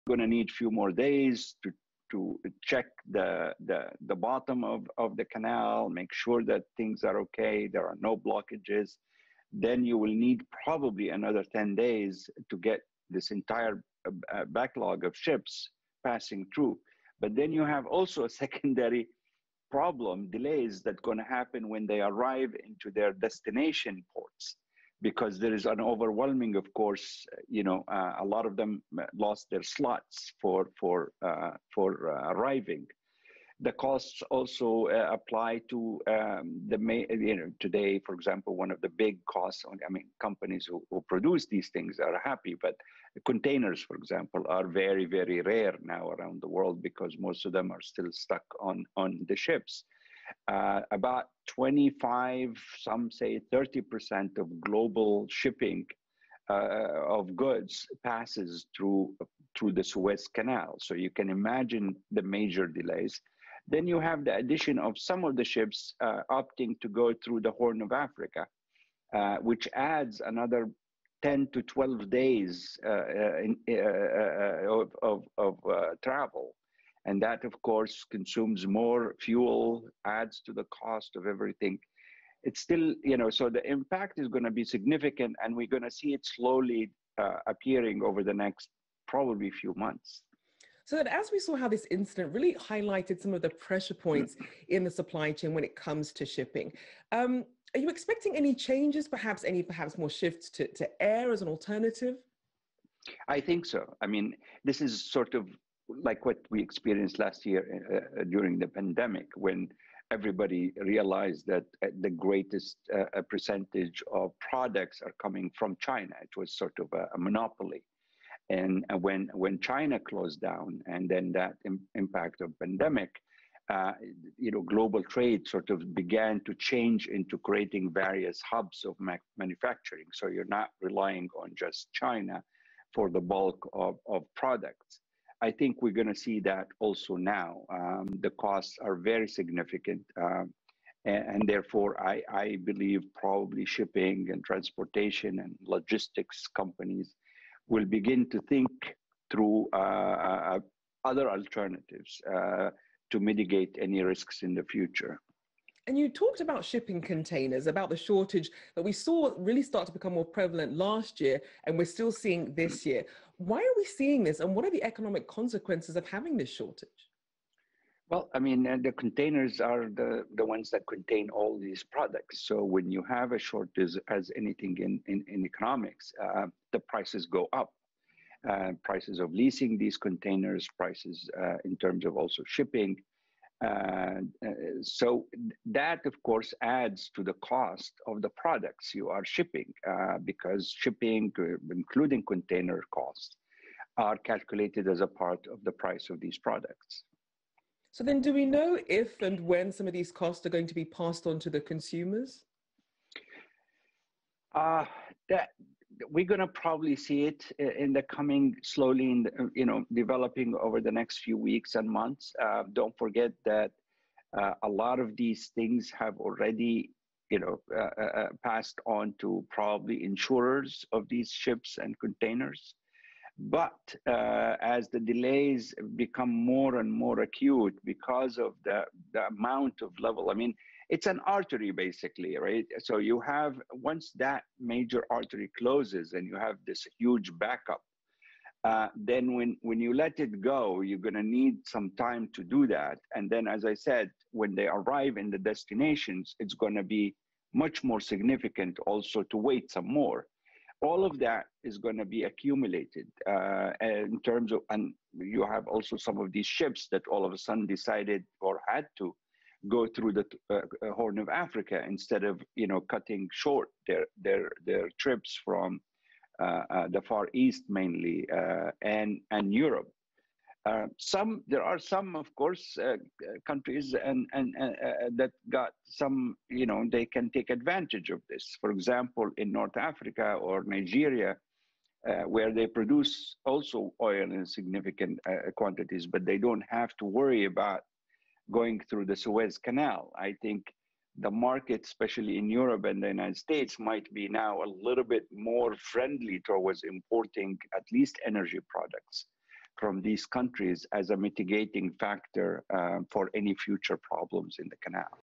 Hafed Al Ghwell on global shipping issues
CGTN's Rachelle Akuffo speaks with Hafed Al Ghwell, senior fellow at John Hopkins' School of Advanced International Studies, on recent shipping problems.